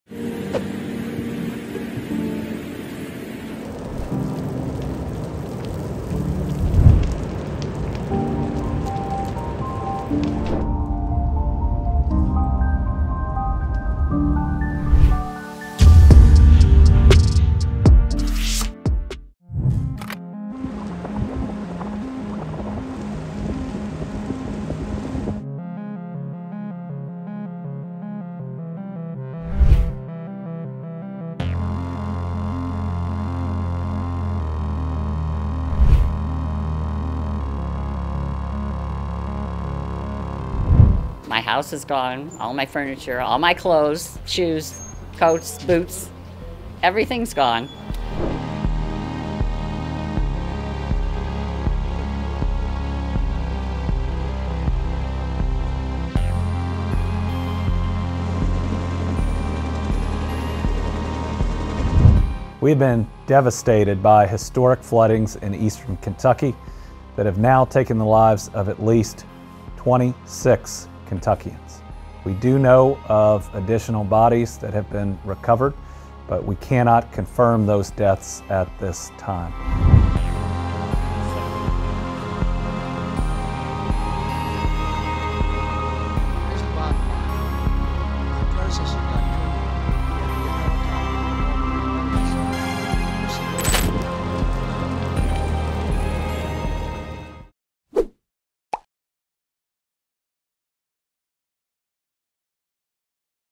서울면적 1/3 불타고, 홍수에 수십명 사망…미국, 기후재앙 몸살 / 연합뉴스 (Yonhapnews)
서울면적 1/3 불타고, 홍수에 수십명 사망…미국, 기후재앙 몸살
    (서울=연합뉴스) 미국 서부가 활활 타오르고 있습니다.
    AP통신 등에 따르면 지난달 29일(현지시간) 미국 캘리포니아주 클래머스 국유림에서 발생한 산불이 빠르게 확산해 이틀 만에 약 208㎢를 태우고 인근 오리건주까지 위협하고 있습니다.
    서울시 전체 면적(605㎢)의 1/3 이상이 잿더미가 된 겁니다.
    이는 올해 캘리포니아에서 발생한 산불 가운데 가장 규모가 크다고 통신은 전했습니다.
    산불은 캘리포니아주뿐 아니라 몬태나와 아이다호 등 서부 곳곳에서 잇따라 발생하고 있습니다.
    미국 동부는 기록적인 폭우로 신음하고 있습니다.
    지난달 27일 켄터키주 고원지대에서 발생한 갑작스러운 폭우로 최소 26명이 숨진 가운데 인근 버지니아주 등으로 홍수 피해가 확산하고 있습니다.
    켄터키 일대 1만8천 가구에 전기와 가스 공급이 중단된 상태입니다.
    조 바이든 대통령은 켄터키에 연방 재난지역을 선포하고 긴급 구호 자금 지원에 나섰습니다.
    미국에서는 최근 기후 변화의 영향으로 기상 고온을 비롯한 기상 이변이 속출하고 있습니다.
    영상 보시겠습니다.

    제작 : 진혜숙·김현주
    영상 : 로이터·Amber Elise Carter·@spriteer_774400 트위터·@ReedTimmerAccu 트위터·@creatingerica 트위터·@realzaidzayn 트위터·@weatherdak 트위터·@Earth42morrow 트위터